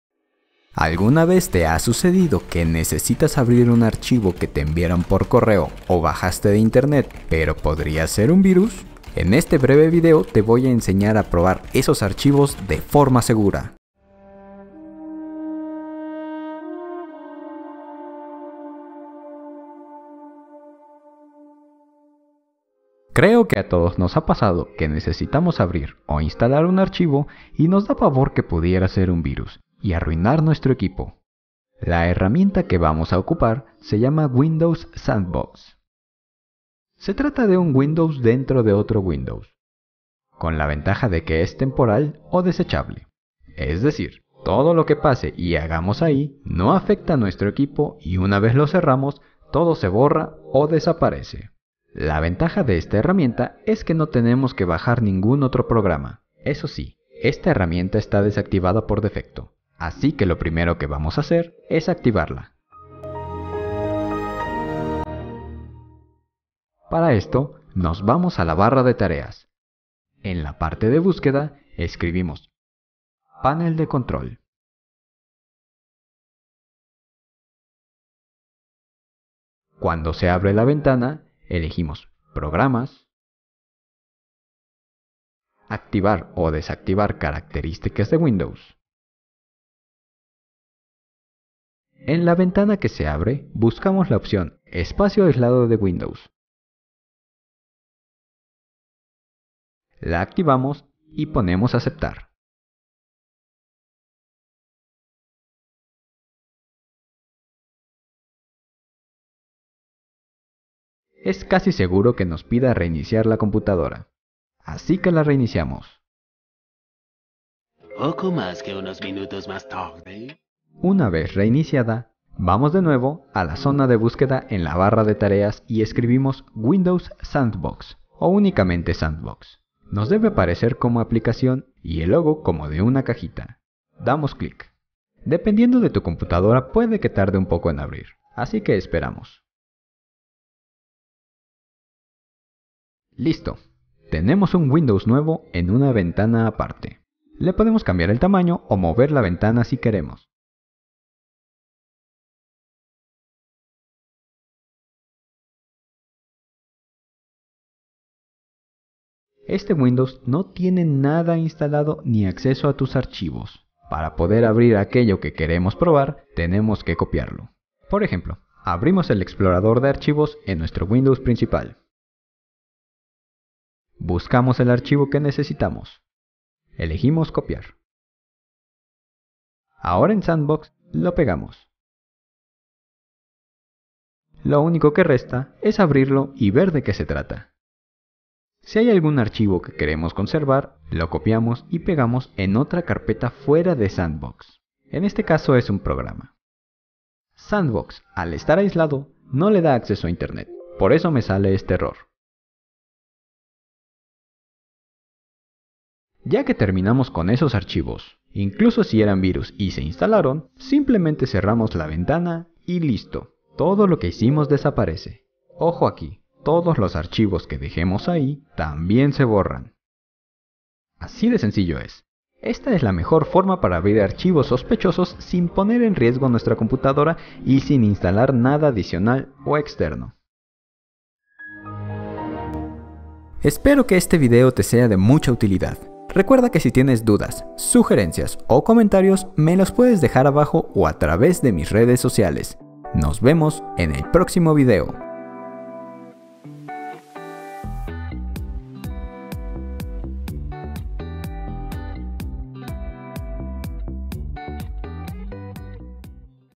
Abrir archivos de forma segura con WINDOWS SANDBOX
En este video te muestro de forma breve como abrir archivos de forma segura usando WINDOWS SANDBOX. De esta forma mantienes tu computadora segura.